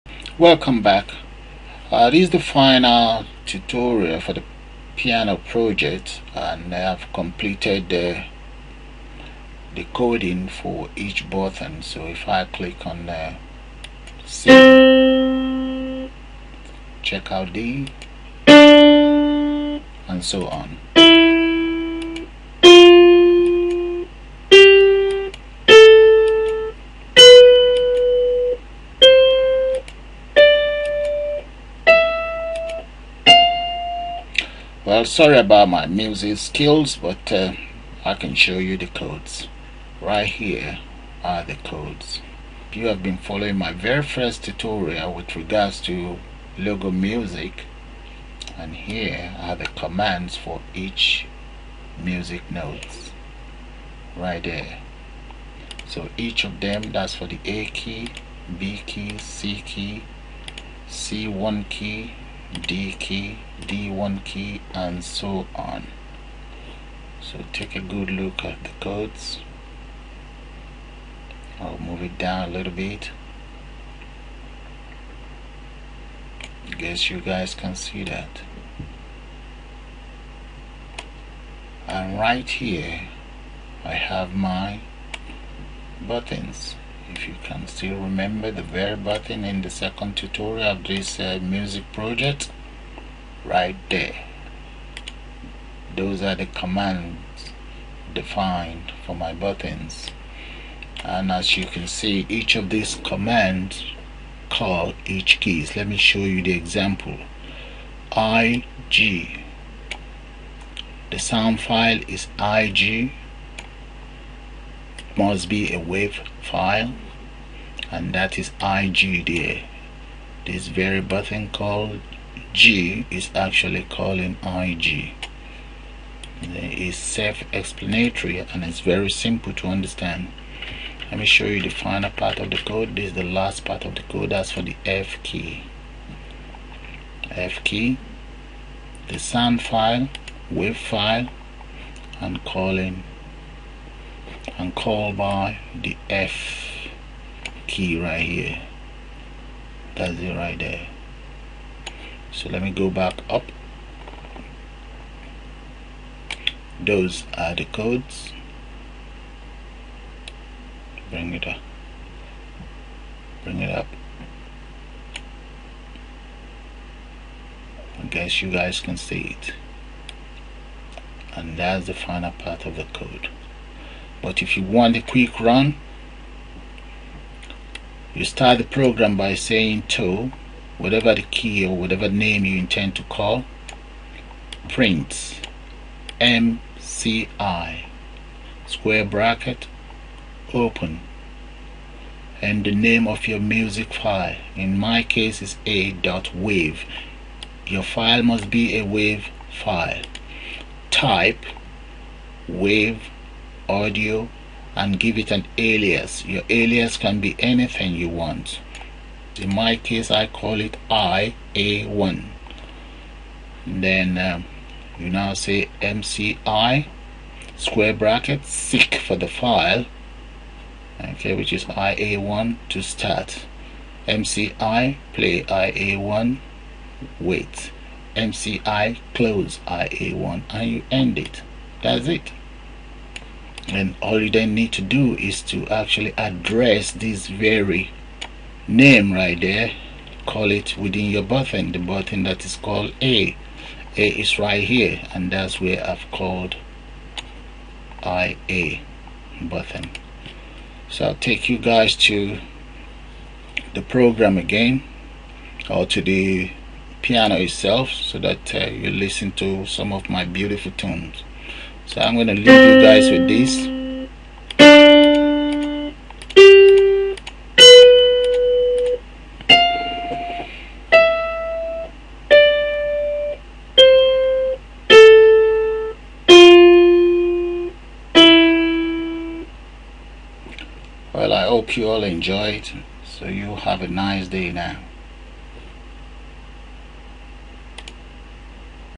How to Create a Piano in MSW Logo Programme
This is the completed version of how to Create a Piano in MSW Logo Programme. Skills you will gain also include how to create buttons for any GUI project.

How to Create Buttons for a Piano in MSW Logo Programme